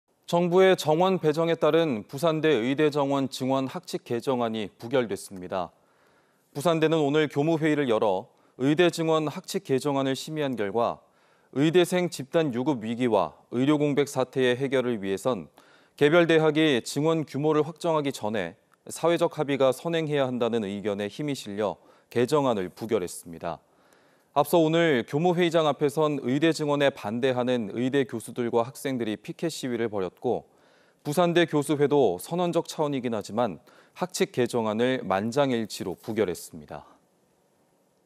부산대, 의대 증원 학칙개정안 부결 / KNN
정부의 정원 배정에 따른 부산대 의대정원 증원 학칙개정안이 부결됐습니다.